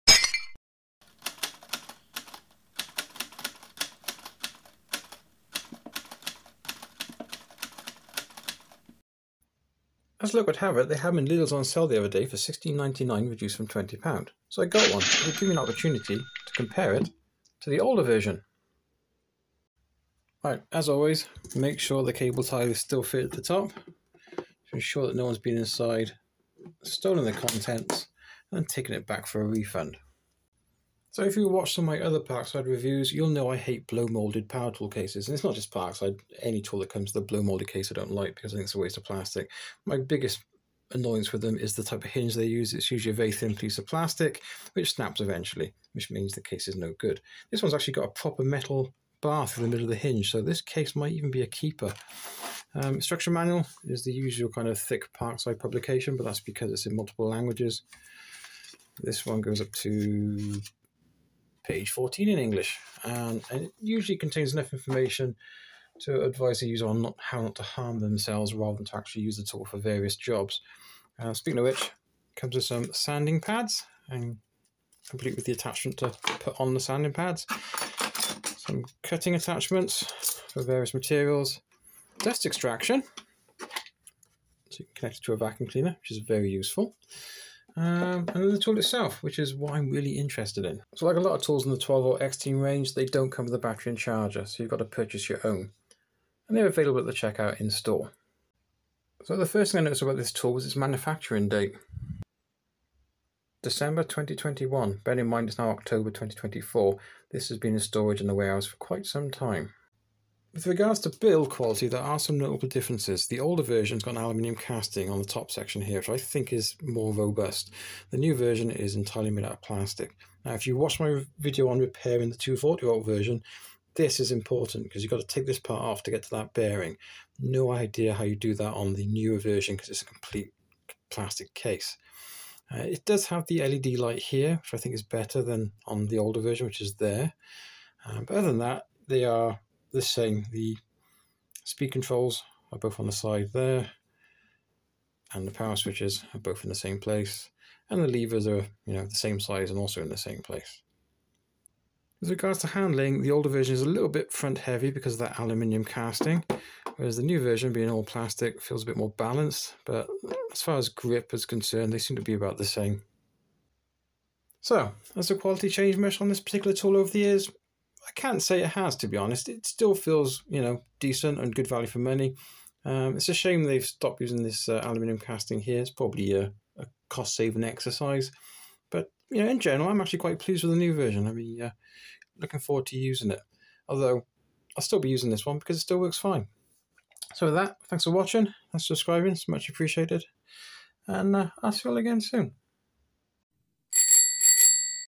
Parkside tools. Is quality getting better, worse or still the same?🤔
This video is a comparison of the old Pre X Team 12v multi tool from 2017 against a 12v X Team compatible multi tool produced from December 2021. Personally I think the eco system X Team range of 12v tools was a good move as the older 12v tools (of which I had 3) all had similar but incompatible 12v batteries.
  The design of the older multi tool was more robust in places though but there's is an X Team model produced between these products that has the aluminum casting so is probably the best of both designs.

Lidl and Park side logo and brand names used with respect of the owners.